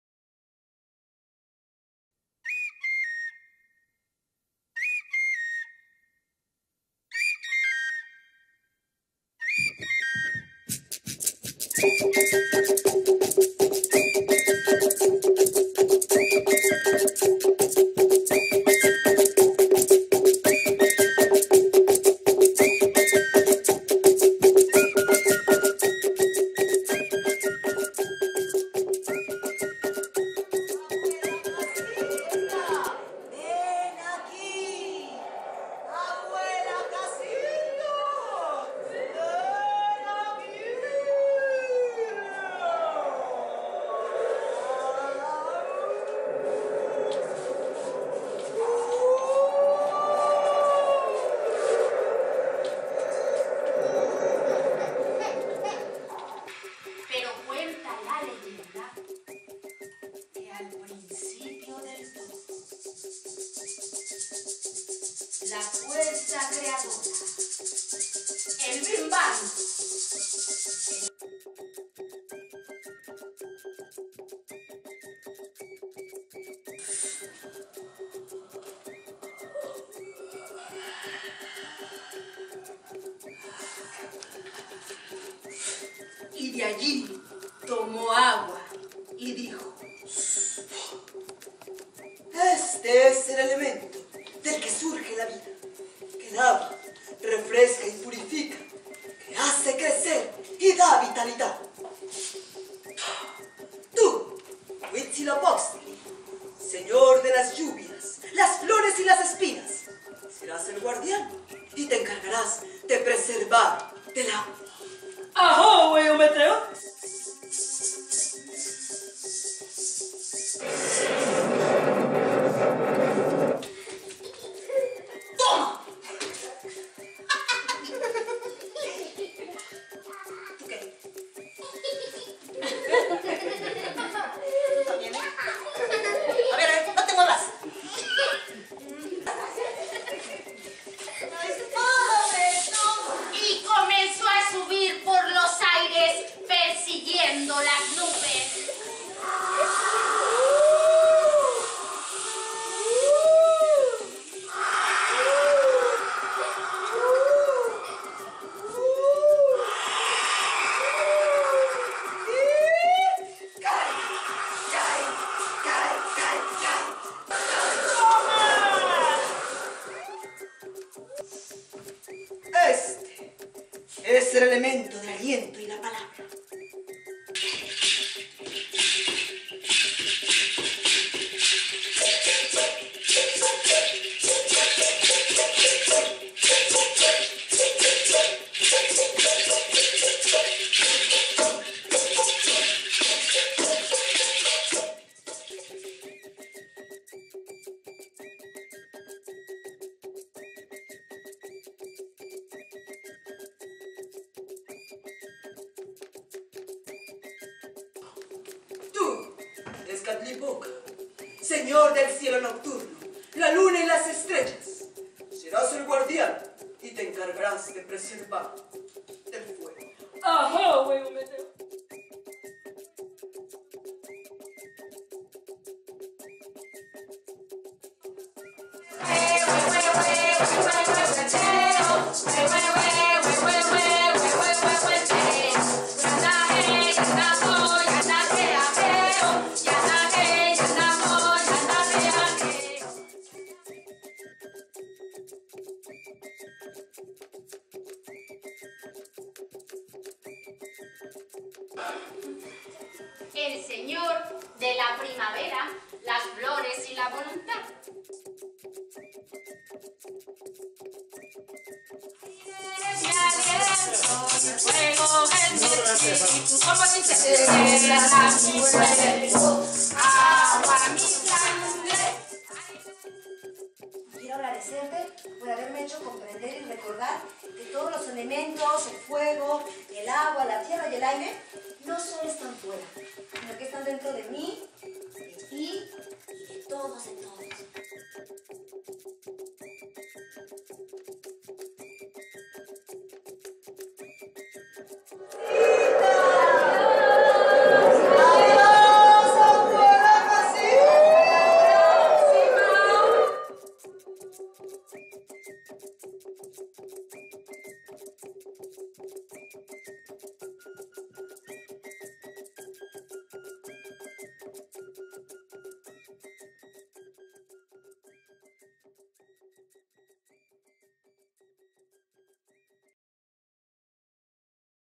Trailer Tlalticpac
Espectáculo escénico familiar, basado en la leyenda de la creación de los elementos.Con música y canto en directo.